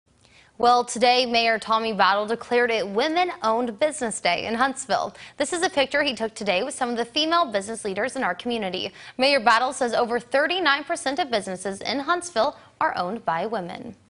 Mayor Battle Declares Women-Owned Business Day in Huntsville
Mayor Tommy Battle declared it women-owned business day in Huntsville Friday.